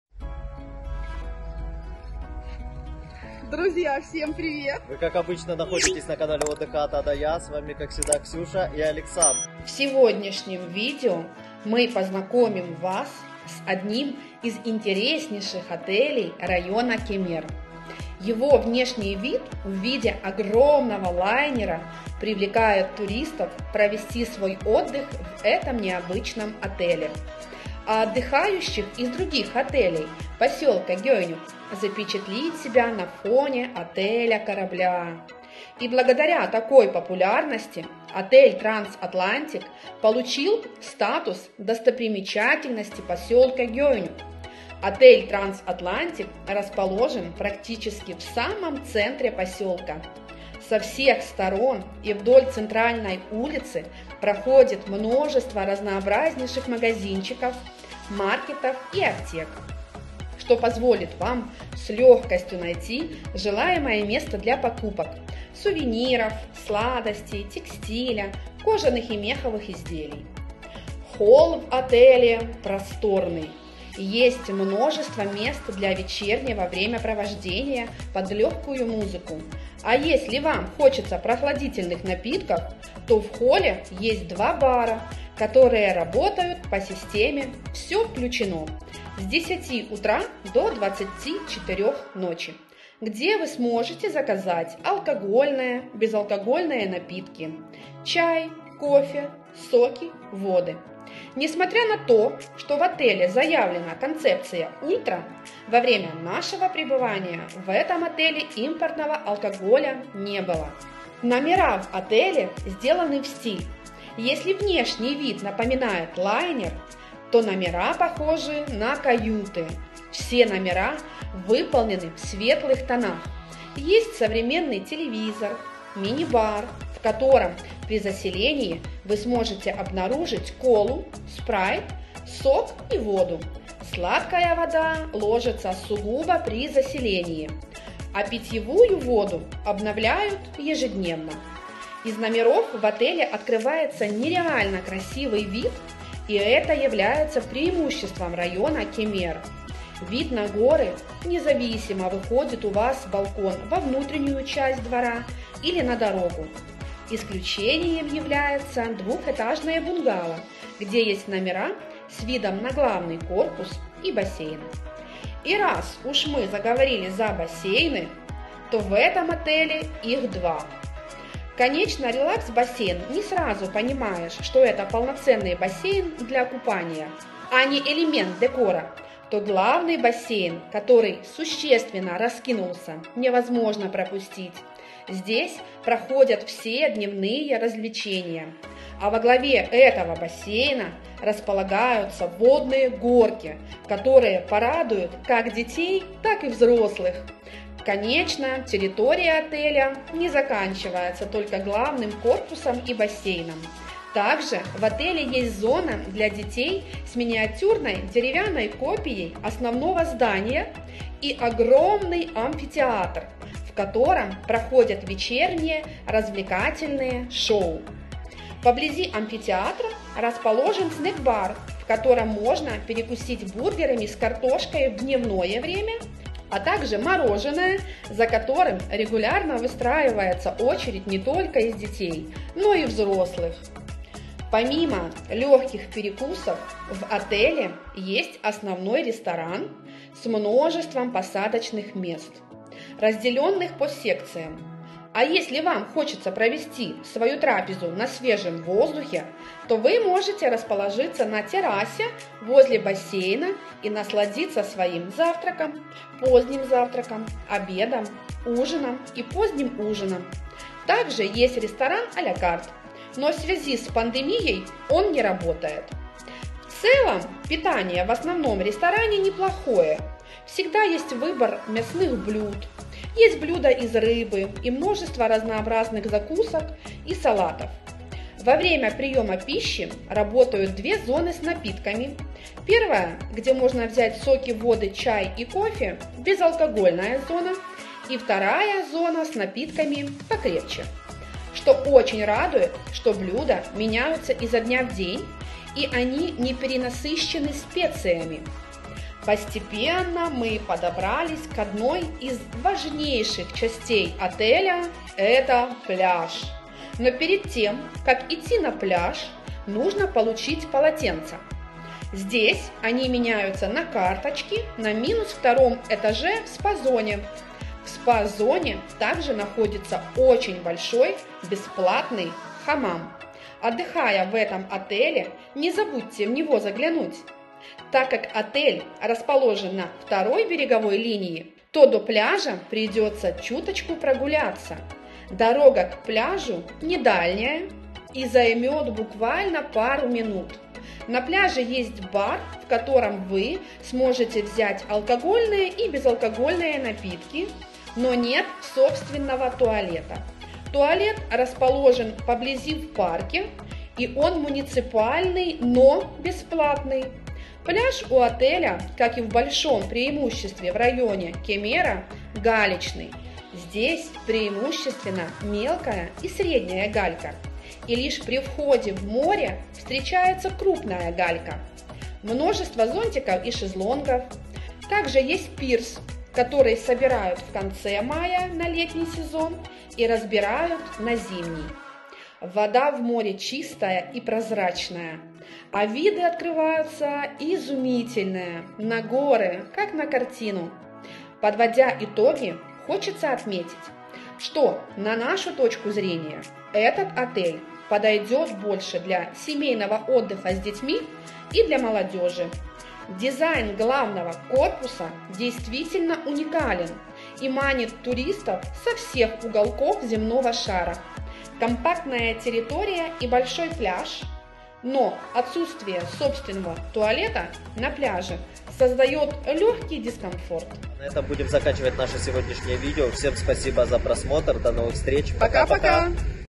Турция 2021. Самый ОРИГИНАЛЬНЫЙ отель в КЕМЕРЕ! Все ХОТЯТ в него! Trans Atlantik 5*
В этом видео мы познакомим Вас с одним из самых оригинальных отелей в Кемере! Отель Trans Atlantik 5* расположен в поселке Гёйнюк! Покажем чем же этот отель так привлекает туристов из разных уголков планеты! Каждый кто хоть единажды слыхал за отель Транс Атлантит хочет в него поехать!

В этом видео покажем номер в отеле, территорию, расскажем про питание и бары! Ну и конечно же прогуляемся на пляж и покажем за что так любят регион Кемер!

✈️donat на путешествия (monobank) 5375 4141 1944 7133